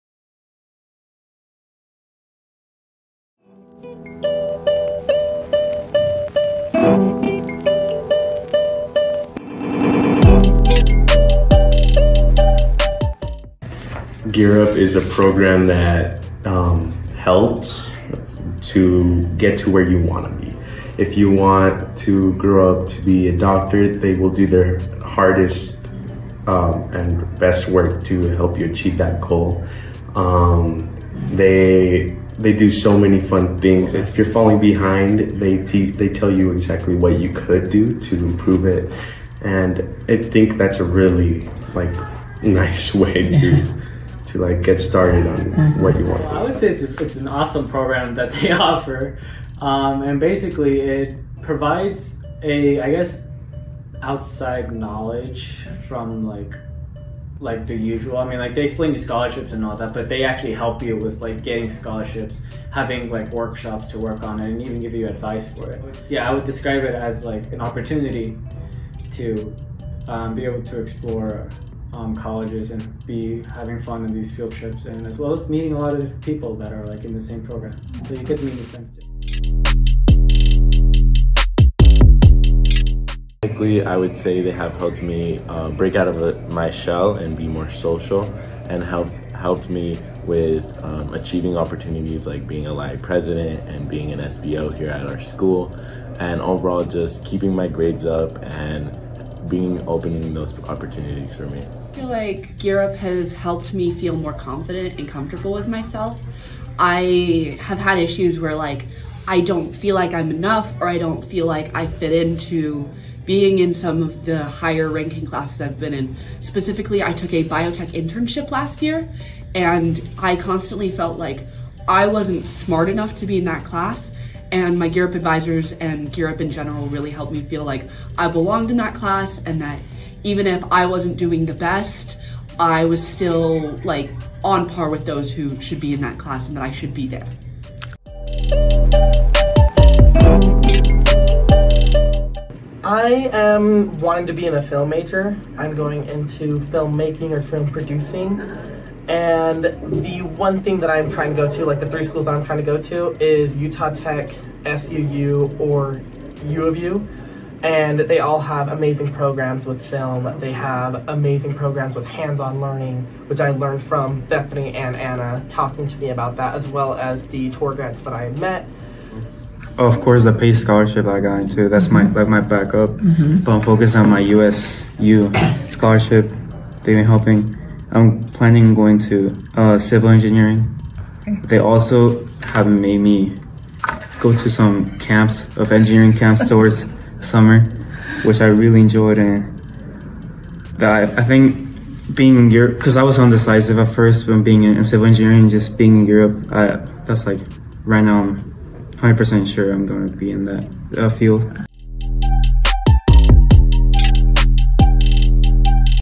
Granite Seniors Share GEAR UP Experiences 2024
Graduating high school Seniors from the Granite School District share their experiences through the GEAR UP program, along with the opportunities that GEAR UP has provided them for the future.

GEAR UP, which stands for Gaining Early Awareness and Readiness for Undergraduate Programs, is a national, federally funded grant program designed to increase the number of low-income students who are prepared to enter and succeed in postsecondary education. GEAR UP is a partnerships of schools, institutions of higher education, state agencies, designed to empower partner schools and students.